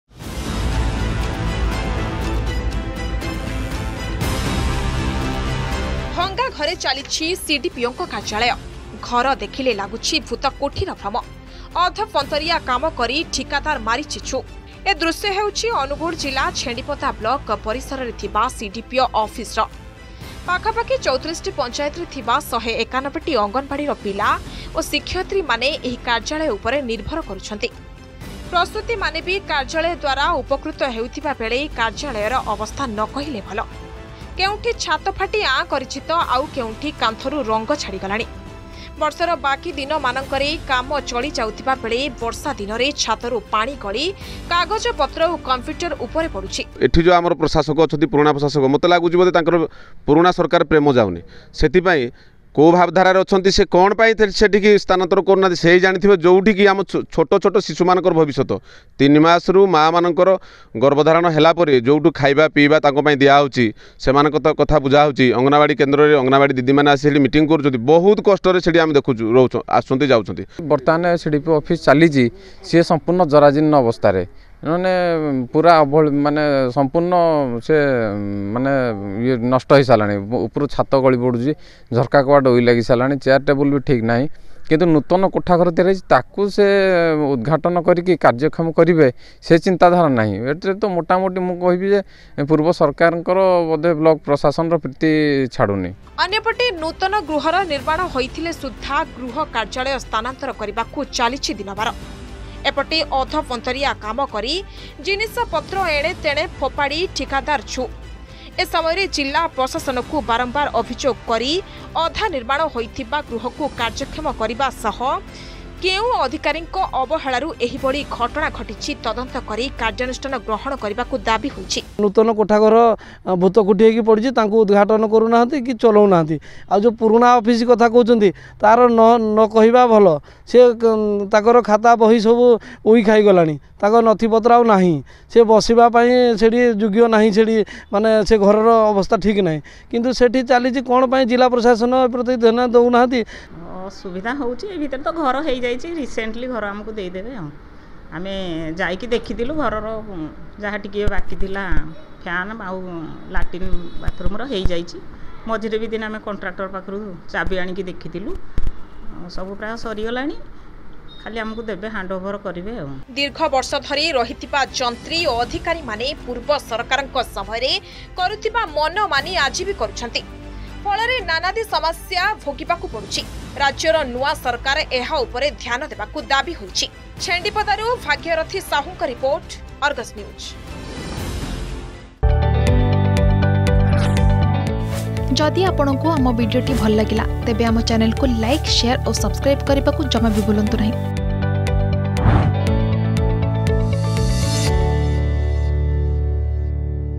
ଭଙ୍ଗା ଘରେ ଚାଲିଛି ସିଡିପିଓଙ୍କ କାର୍ଯ୍ୟାଳୟ || CDPO Office Issue
ଭଙ୍ଗା ଘରେ ଚାଲିଛି ସିଡିପିଓଙ୍କ କାର୍ଯ୍ୟାଳୟ । ଘର ଦେଖିଲେ ଲାଗୁଛି ଭୂତକୋଠିର ଭ୍ରମ । ଅଧପନ୍ତରିଆ କାମ କରି ଠିକାଦାର ମାରିଛି ଛୁ ।